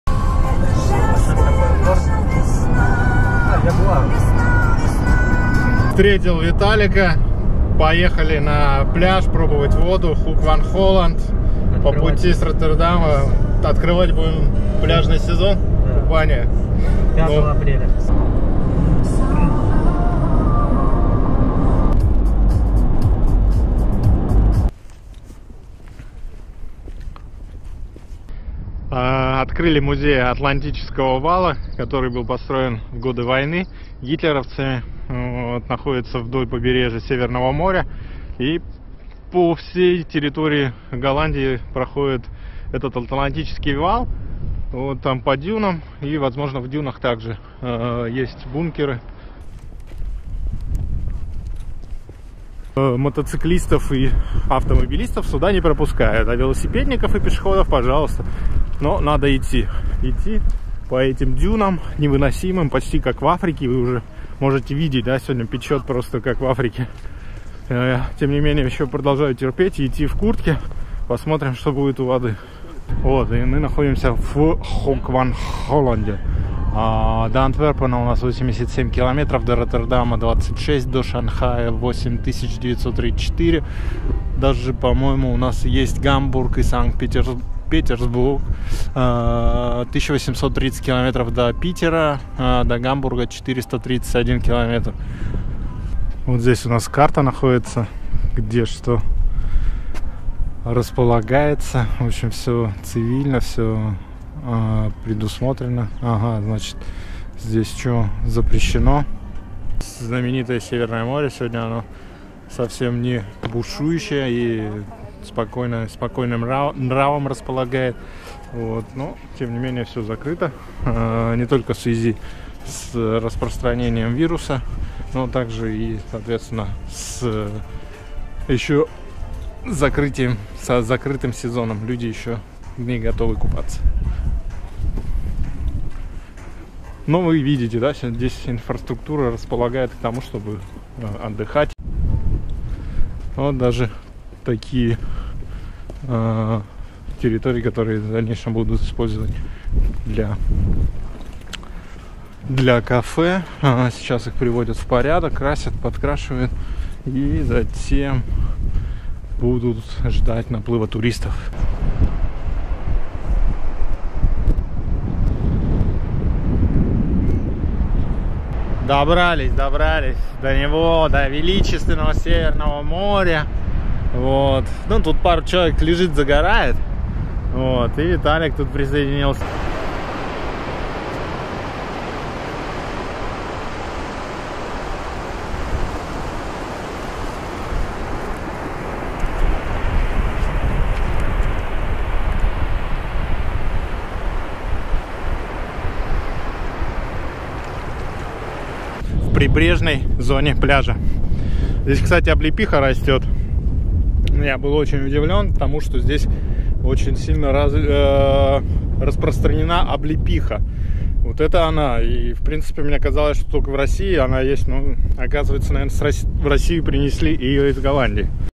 Обычный голландский пляж и бесплатный! Как выглядит не в сезон...
К чему стоит стремится российским властям, чтобы сделать из пляжей на Финском заливе что-то подобное как в Голландии.